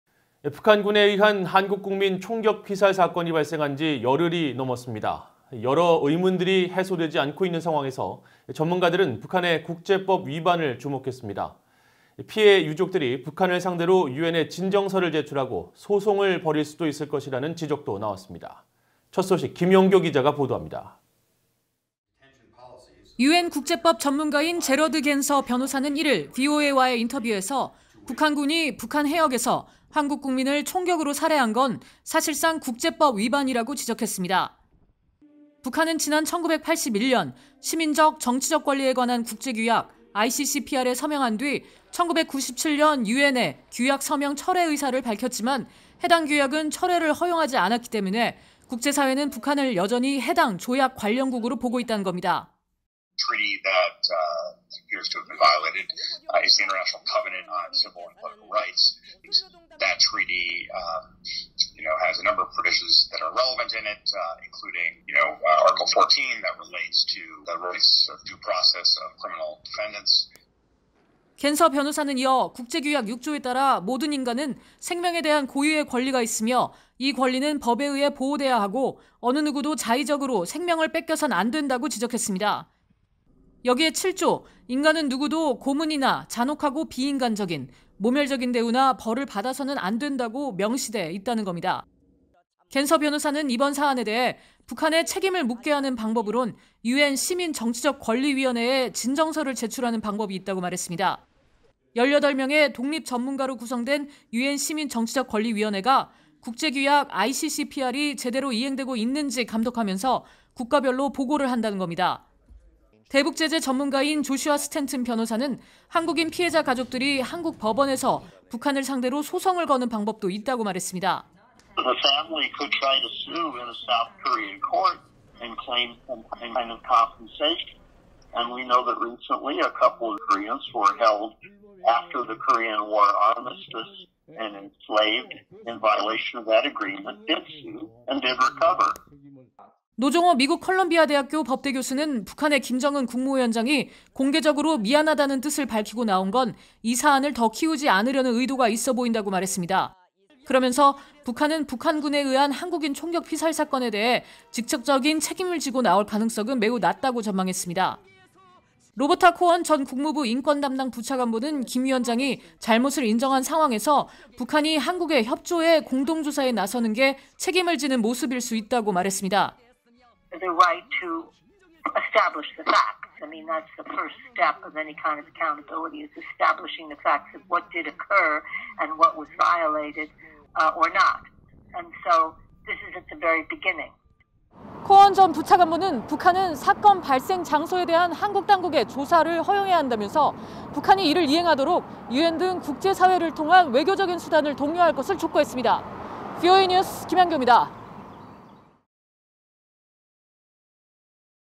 [VOA 뉴스] “북한군 ‘한국 국민’ 사살…‘유엔 진정서·소송’ 방법”
북한군에 의한 한국 국민 총격 피살 사건이 발생한 지 열흘이 넘었습니다. 여러 의문들이 해소되지 않고 있는 상황에서 전문가들은 북한의 국제법 위반을 주목했습니다. 피해 유족들은 북한을 상대로 유엔에 진정서를 제출하고 소송을 벌일 수 있을 것이라는 지적도 나왔습니다. 김영교 기자가 보도합니다. (영상취재: 이상훈 / 영상편집: 조명수)